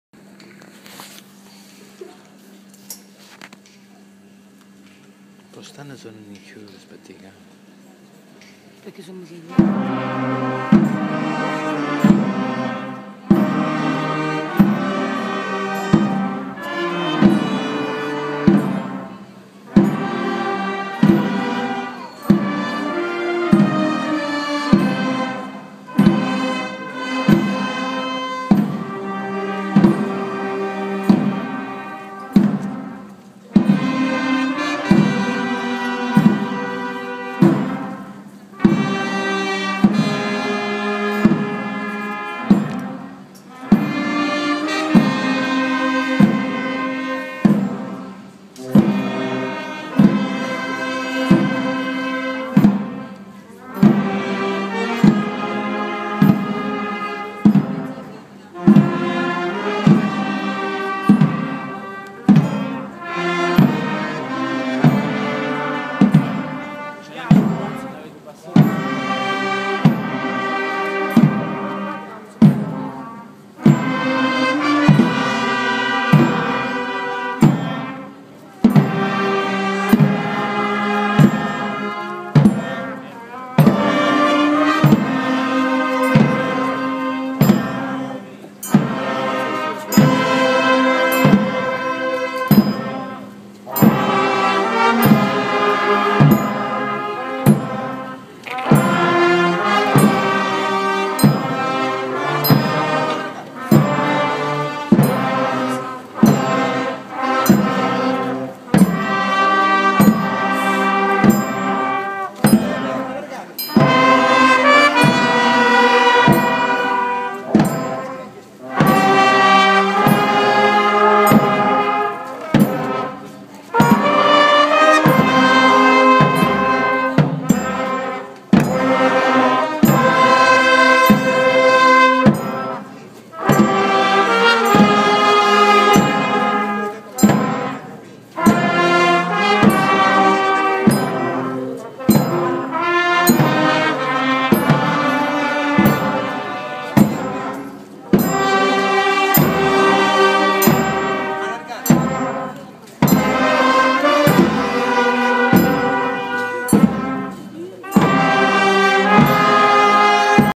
Processione Venerdì Santo CORIGLIANO CALABRO
Processione Venerdì Santo: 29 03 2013
Orchestra di Trombe - Vincenzo Tieri
diretta dal Maestro Vincenzo Greco
Dirigente Scolastico: Aldo Fusaro